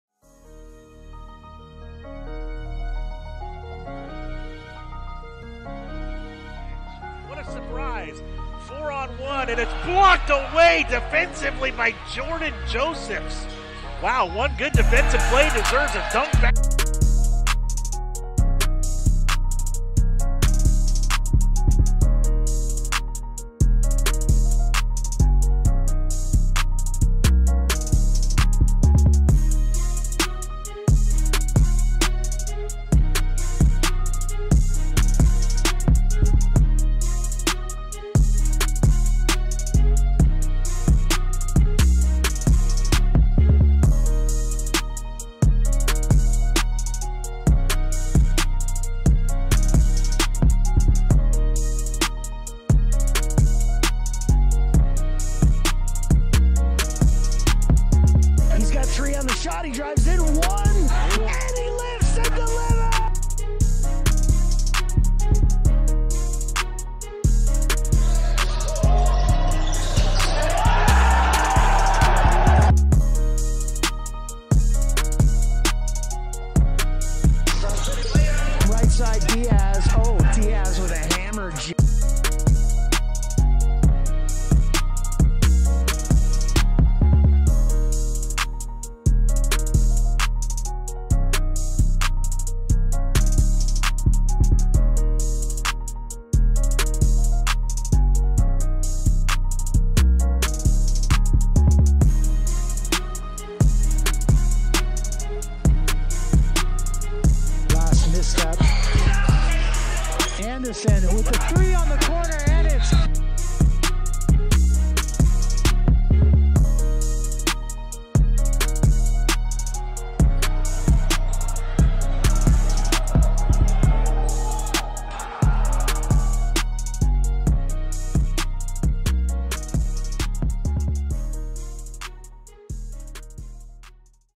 2021-2022 Dominican Men's Basketball Highlights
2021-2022 Season Highlights from Dominican University of California Men's Basketball